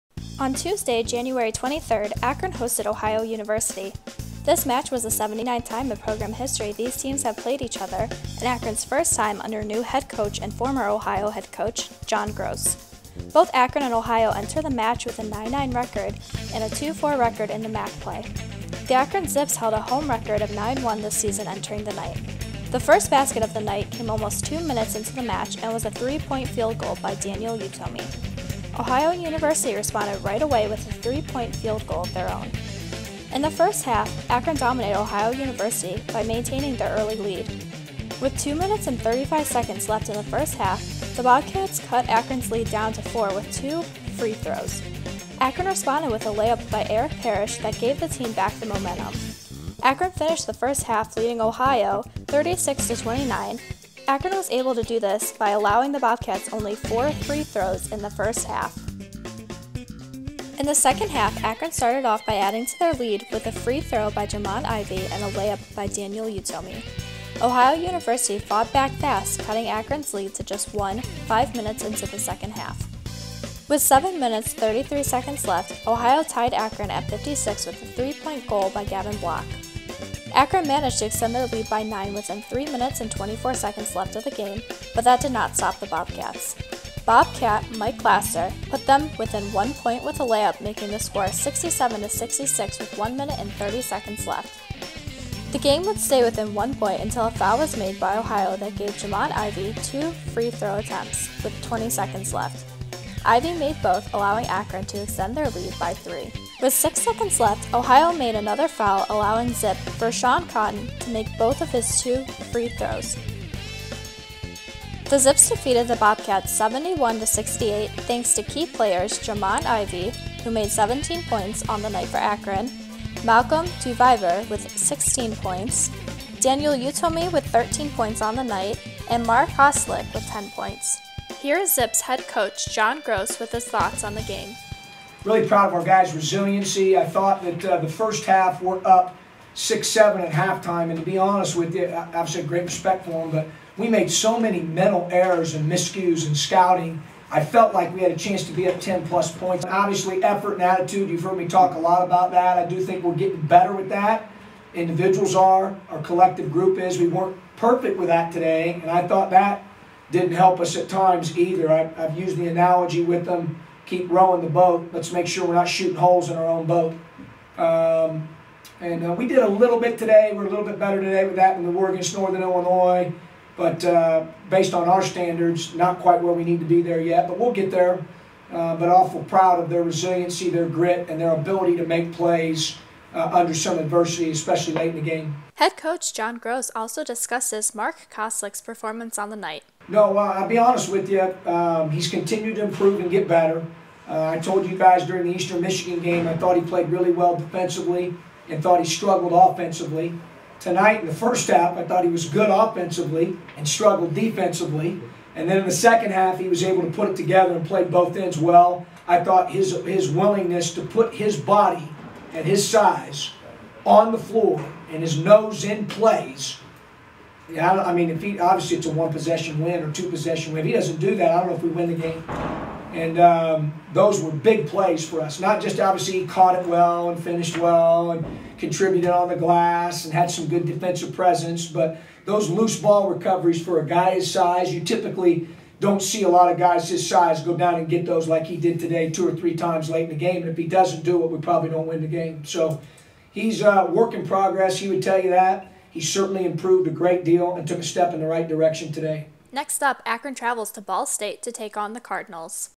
Akron Men's Basketball Vs Ohio University
Camera operator and video editor for the Zips game against the Bobcats.